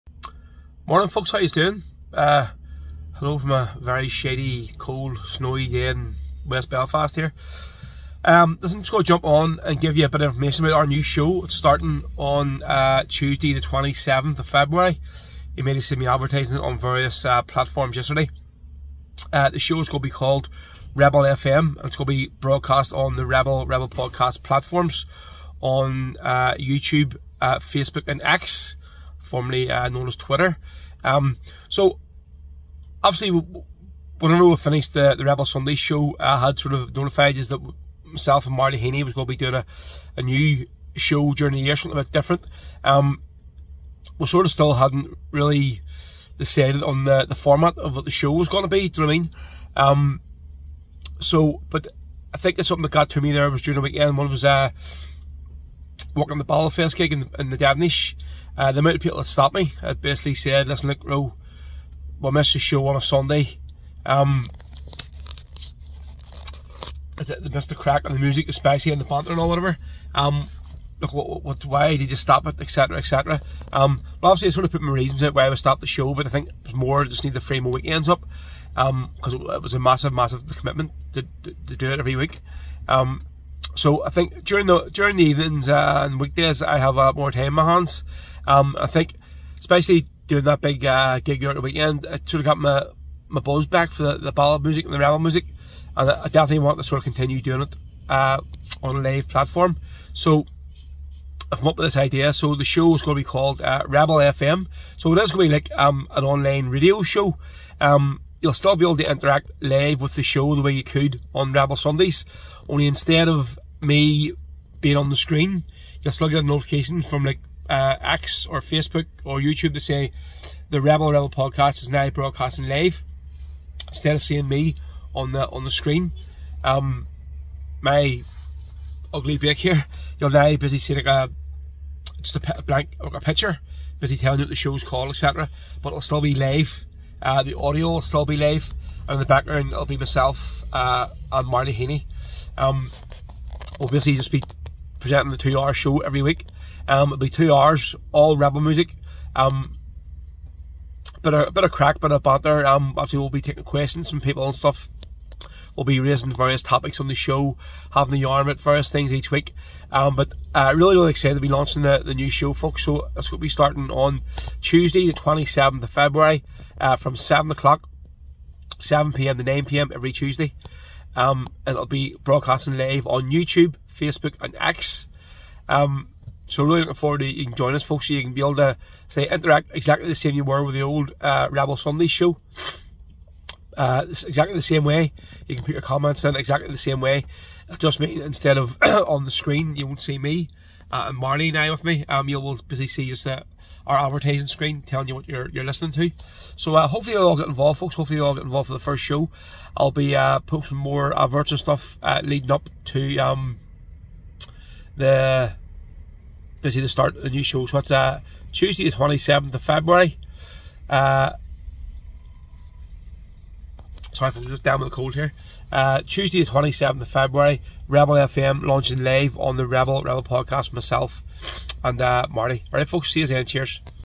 looking forward to launching Rebel FM our brand new Show with myself & Marty ( Spanner )
Launching Tuesday 27th February

Live at 7pm

Rebel fm 📻🎙🎶🇮🇪

Multi Streaming on the Official Rebel Rebel Podcast pages on YouTube, Facebook & X ( Formally Twitter )

Hosted each week by Big Ro & Marty